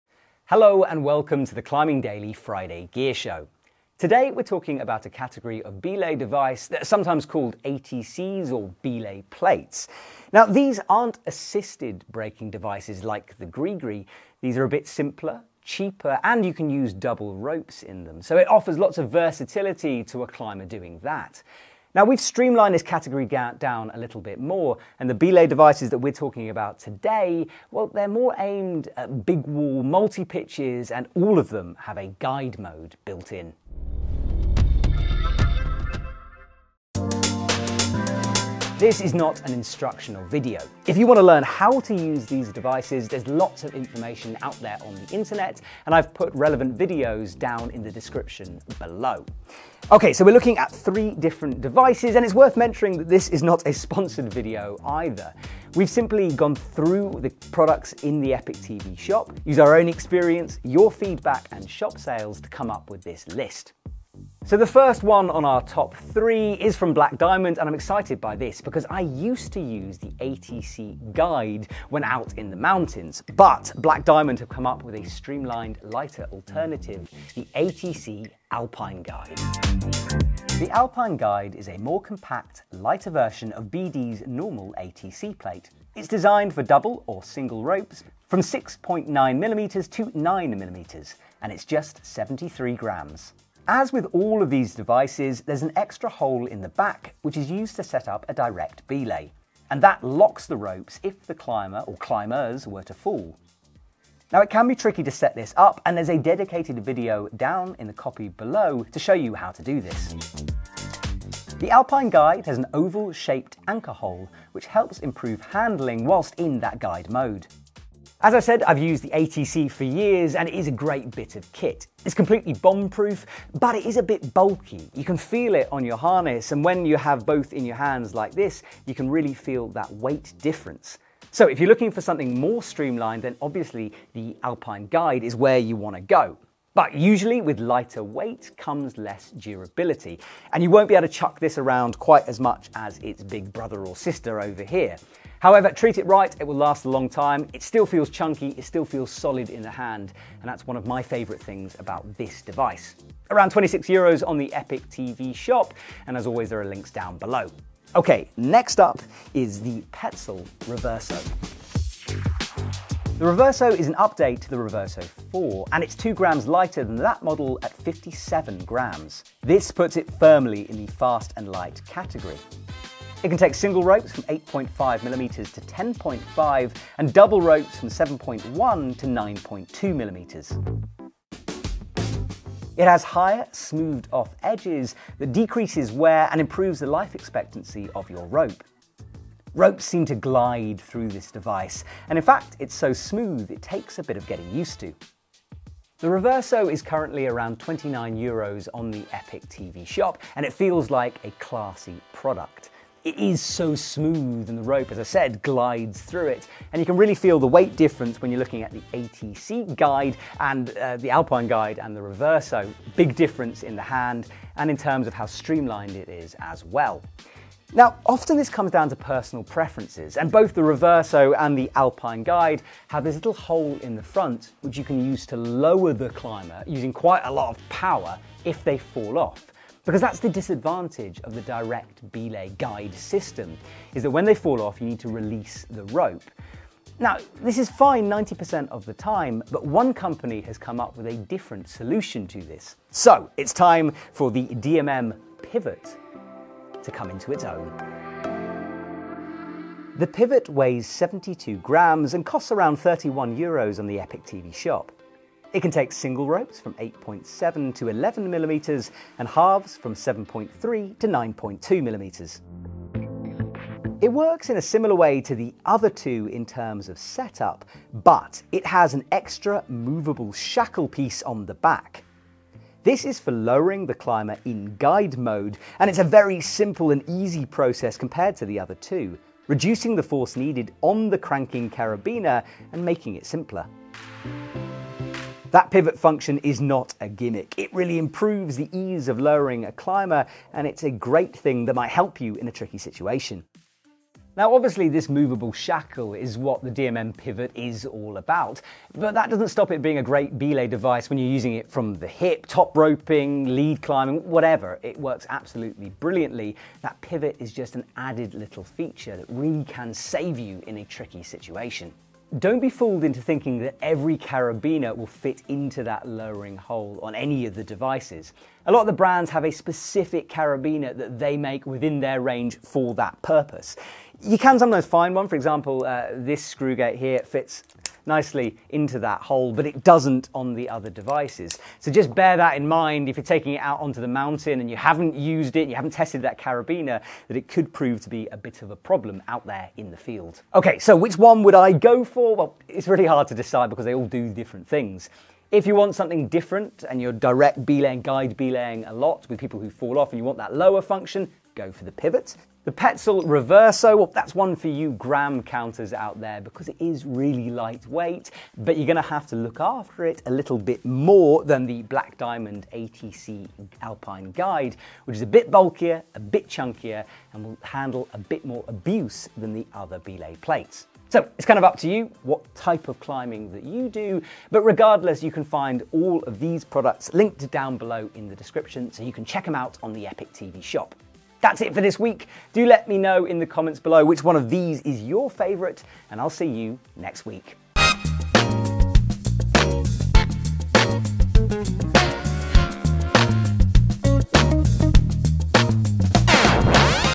The BEST Guide Belay Plates: Top 3 | Climbing Daily Ep.2084
Belay devices come in lots of different shapes and sizes so you can pair them with the type of climbing you want to do. On today's Top 3 show, we check out belay plates designed for multi-pitches and guide mode belaying.